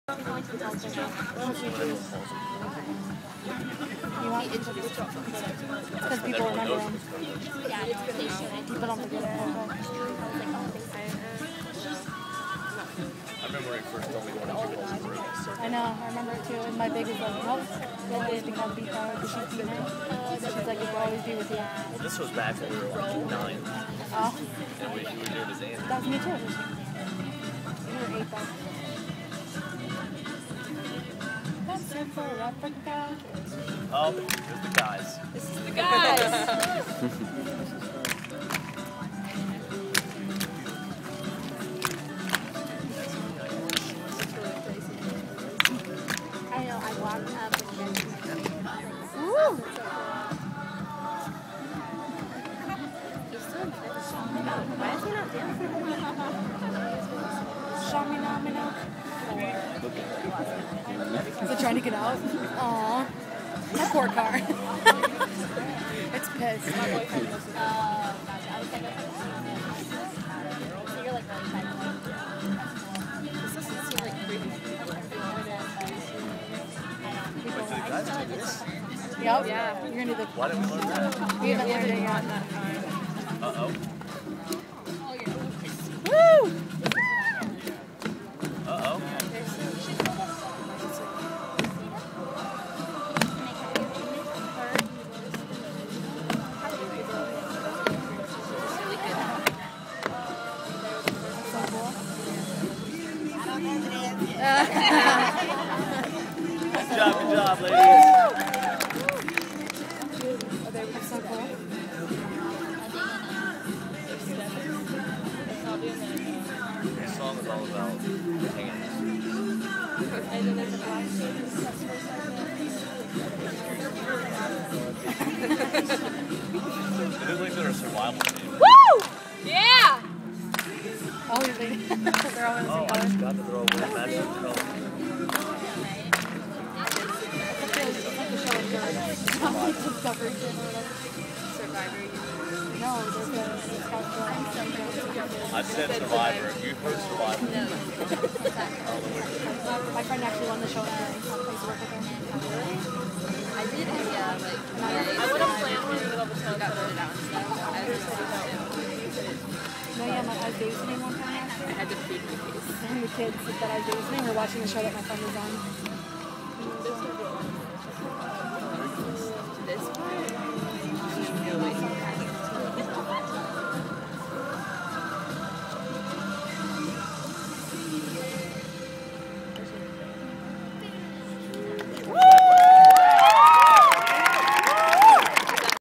UNF Flash Mob (First Choreography)
The Flash Mob committee team shows the dancing choreography for the first time.